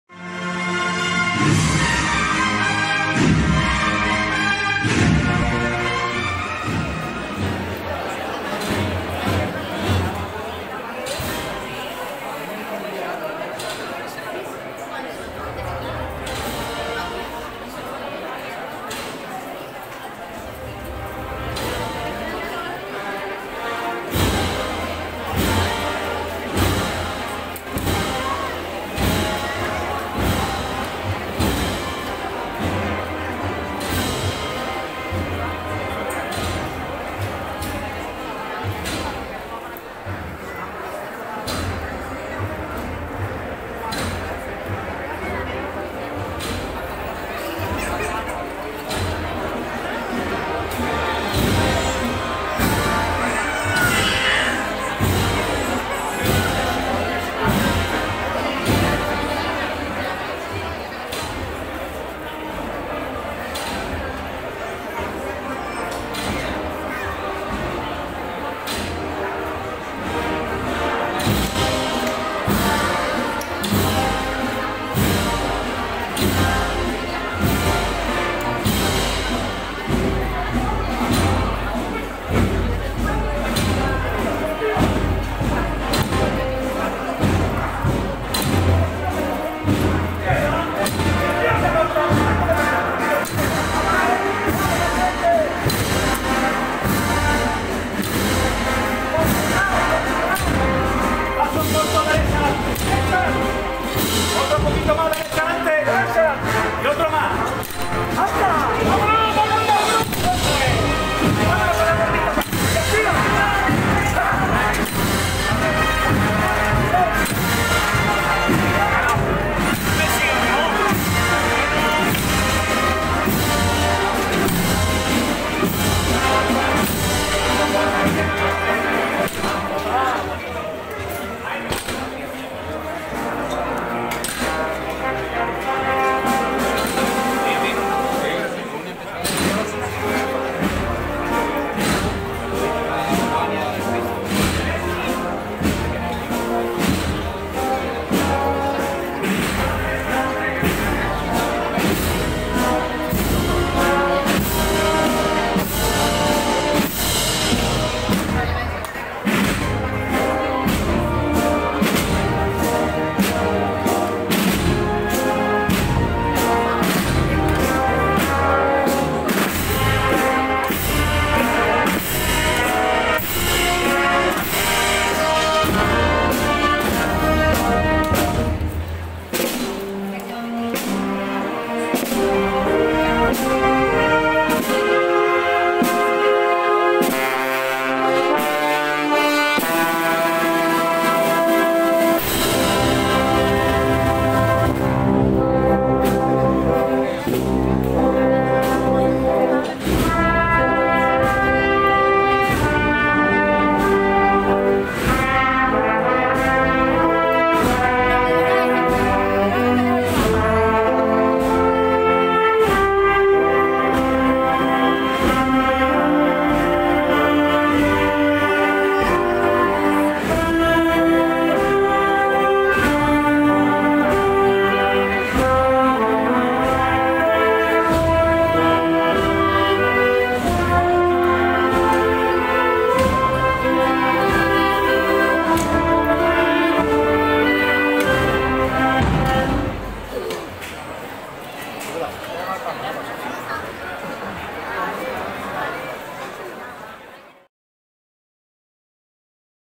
Luz (Las Aguas), San Francisco, Miércoles Santo Cádiz 2022
El paso de palio de Nuestra Señora de la Luz, titular mariana de la Hermandad de las Aguas de Cádiz, a su paso por la calle San Francisco; acompañado por la Asociación Filarmónica Ciudad de Conil, en la noche del Miércoles Santo 13 de abril de 2022.
Colaboración: Juan José Guerrero.